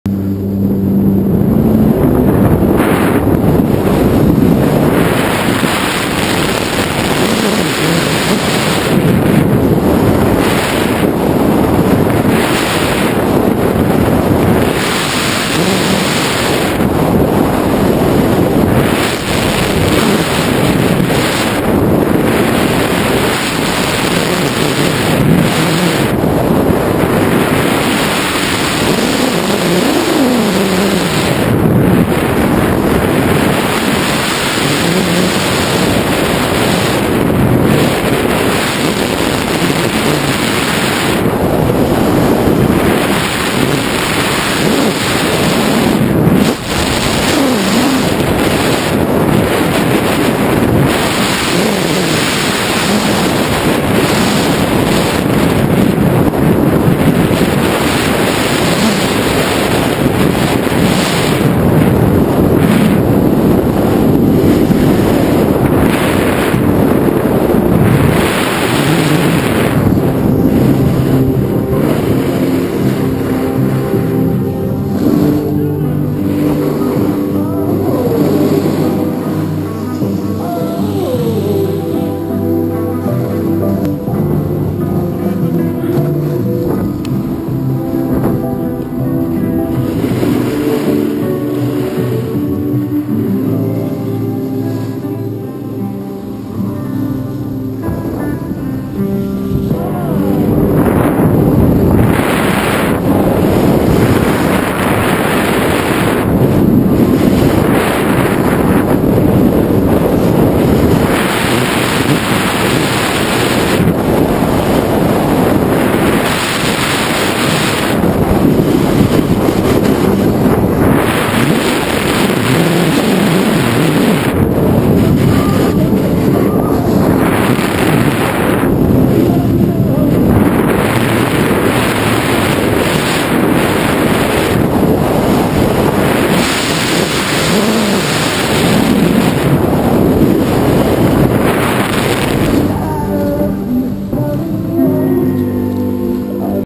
marcelo carrilho - moena - italia - 2014 - led zeppelin - ilaria - carving - gopro
consegui despistar o direito autoral, e vai o video com cortes iniciais e finais - mesmo assim valeu a descida em moena - italia - 2014